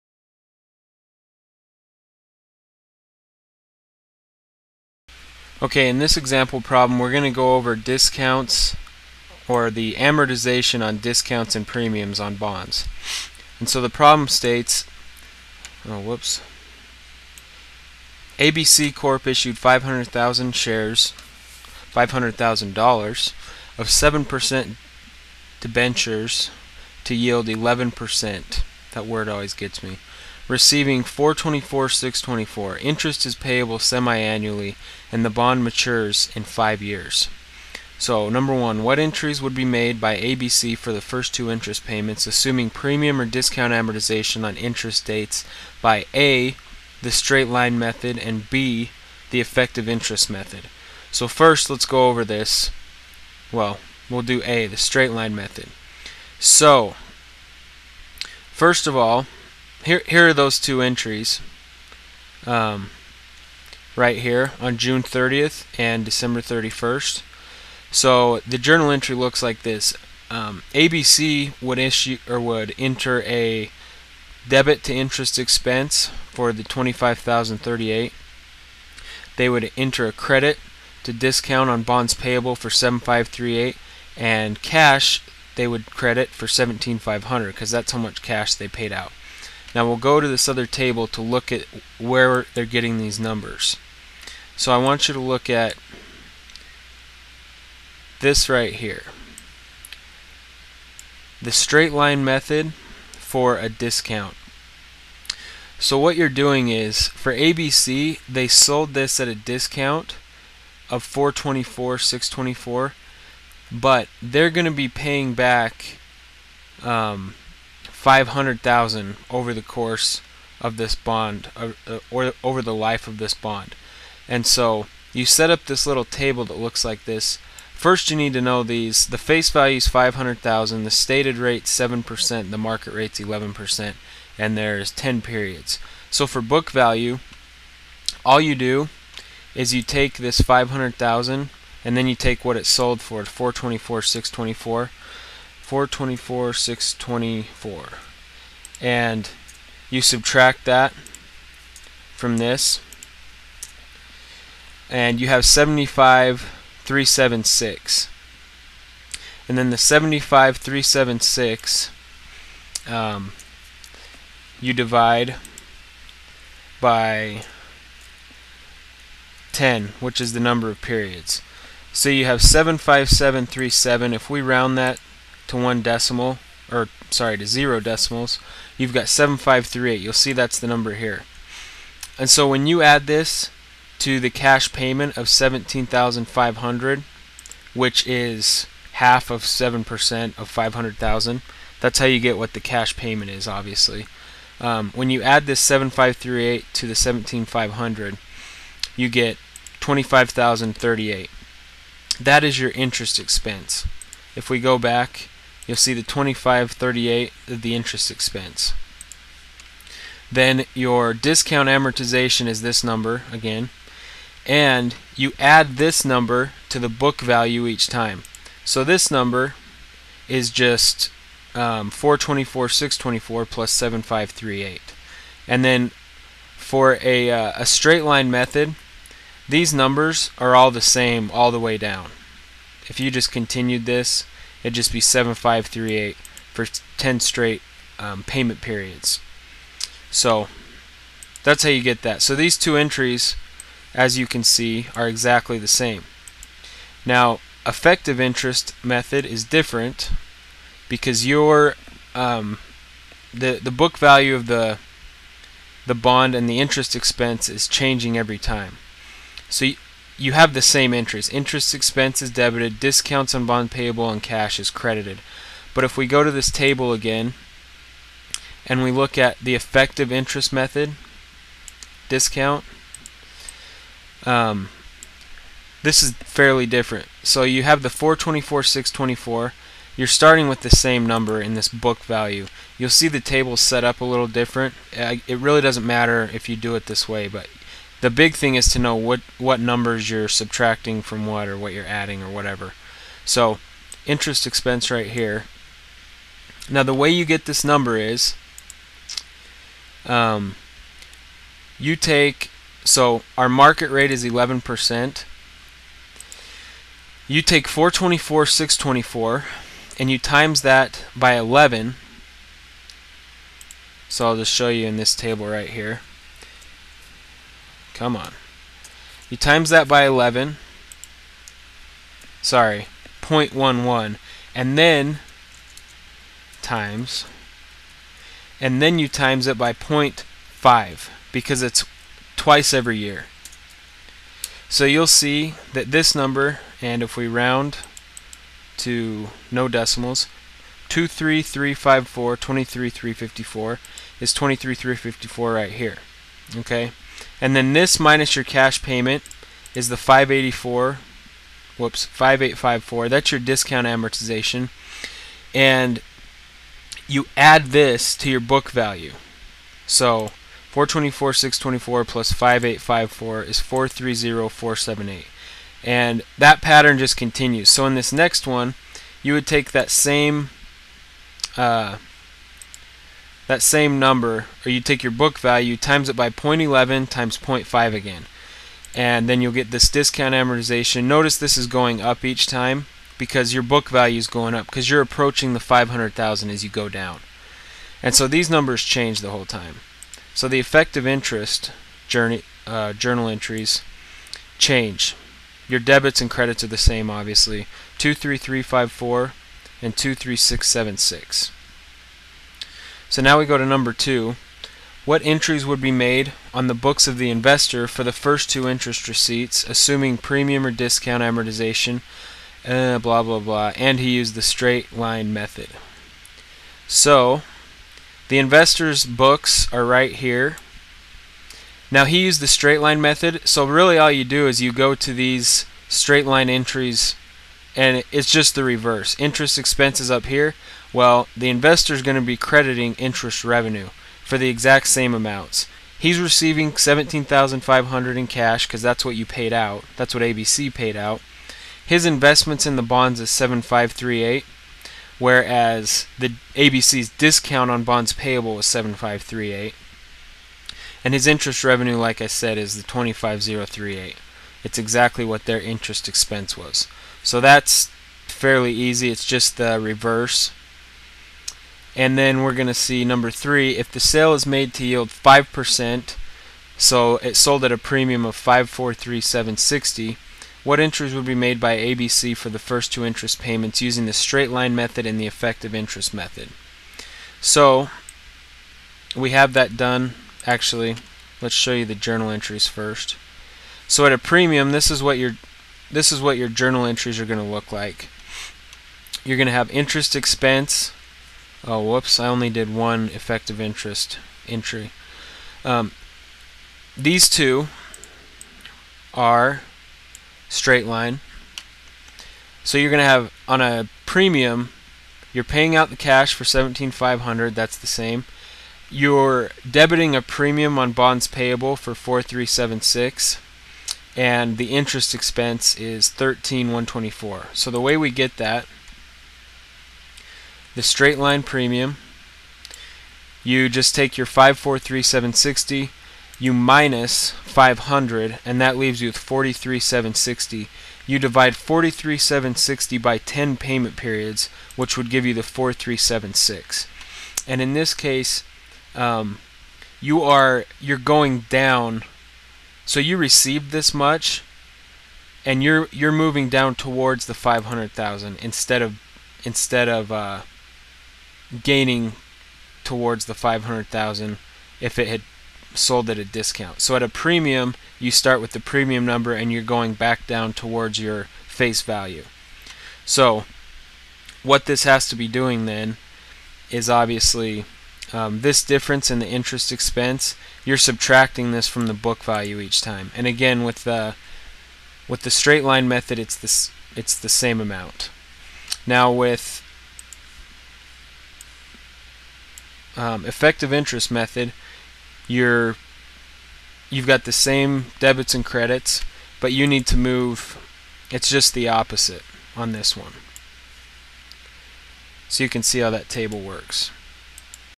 Discount and Premium Amortization on Bonds
An example problem on discount and premium amortization on bonds.